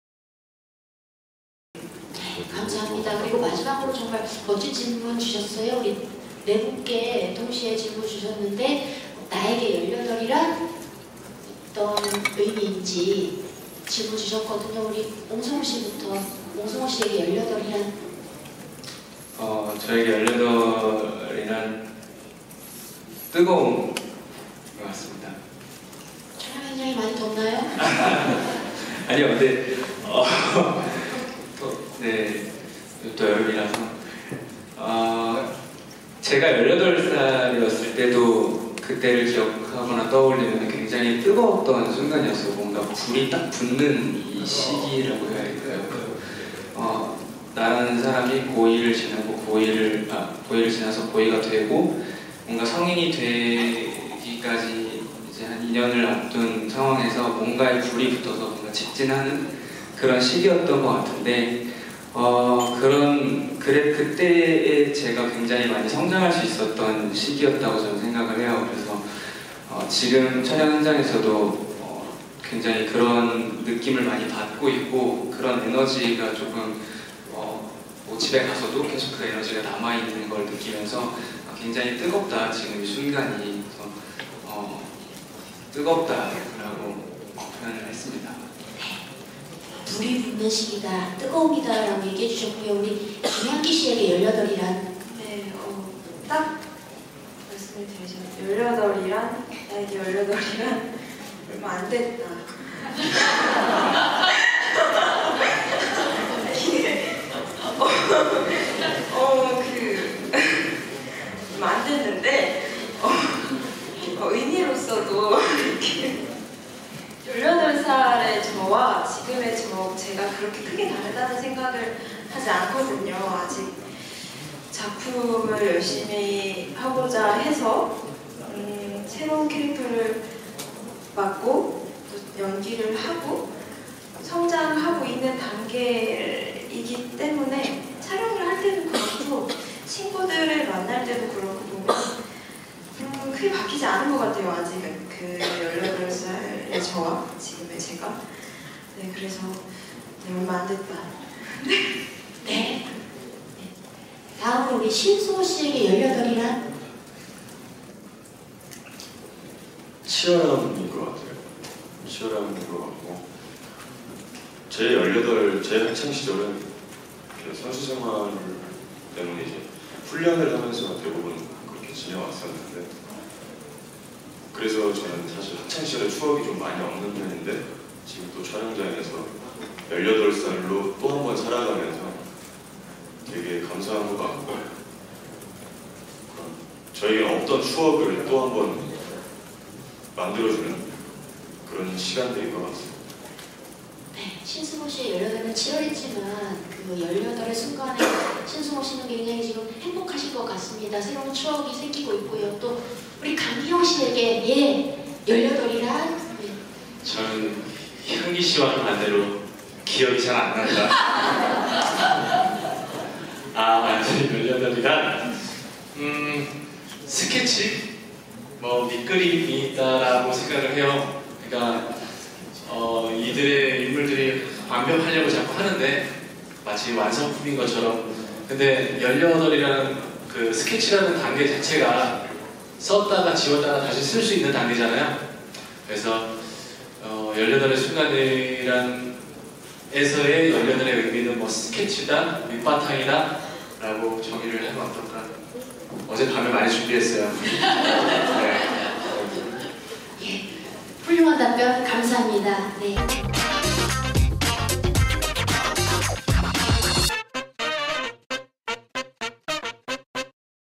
190722 배우들에게 물어본 나에게 18살이란? (JTBC '열여덟의 순간' 제작발표회)
22일 오후 서울시 강남구 논현동 임피리얼 팰리스 서울에서  JTBC 새 월화드라마 '열여덟의 순간' 제작발표회가 열려 배우 옹성우X김향기X신승호X강기영이 나에게 18살 이란? 질문에 답변을 하고 있다.

JTBC 새 월화드라마 '열여덟의 순간'은 위태롭고 미숙한 'Pre-청춘'들의 세상을 있는 그대로 들여다보는 감성 청춘 드라마. 워너원 해체 이후 옹성우의 첫 드라마 주연 작품으로 오늘 22일(월) 오후 9시30분 첫방송된다.